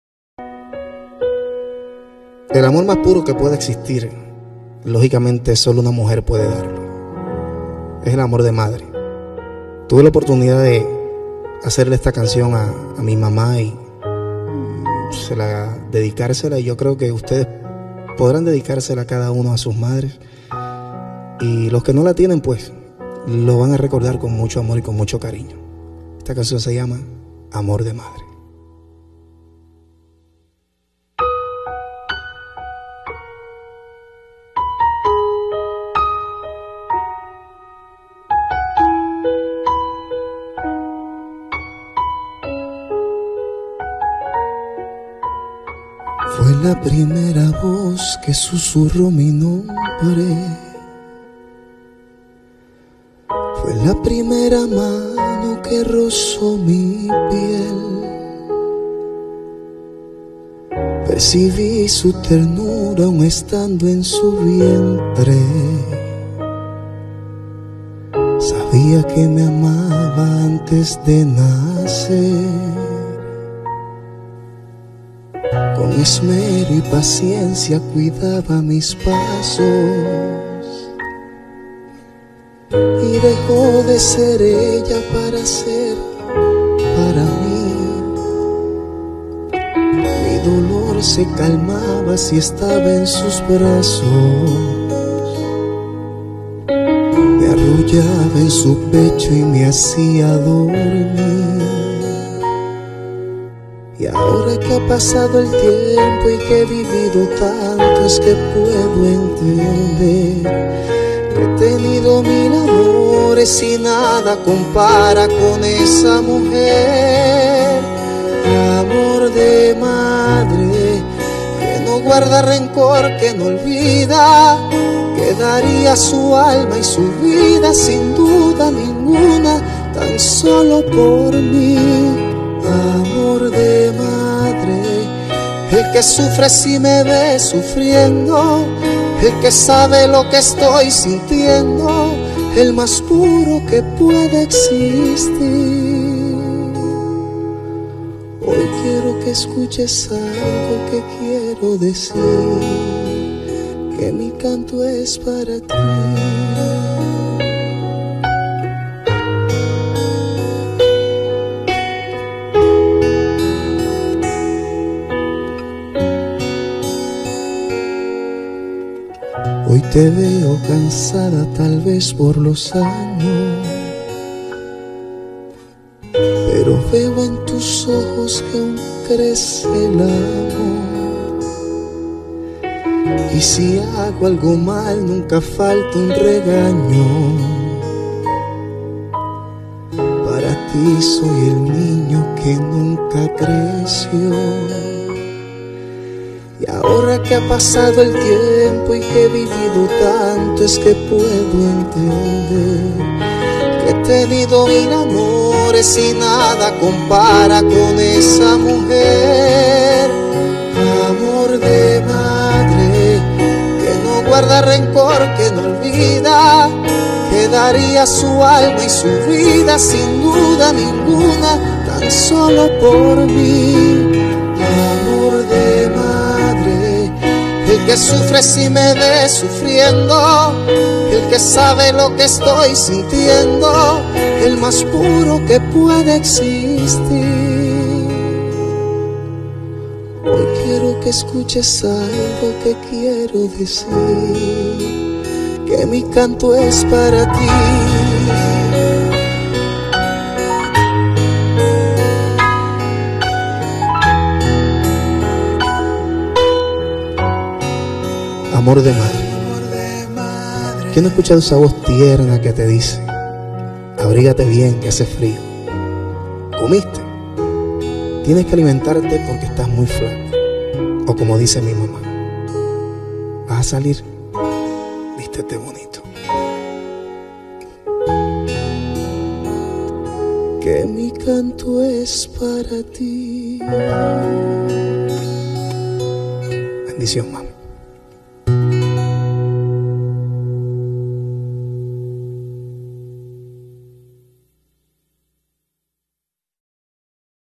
Con Amor Para Mamá... "Amor de Madre"
Amor de madre, el anor mas puro y sincero que podemos experimentar en nuestra vida, ese smor que no importando como seas esta ahi incondicionalnente para ti, para darte apoyo en cada momento de nuestra existencia.. Un lindo homenaje para mi madre ese lindo ser que Dios aun nos da el privilegio de disfrutar, para ella con mucho Amor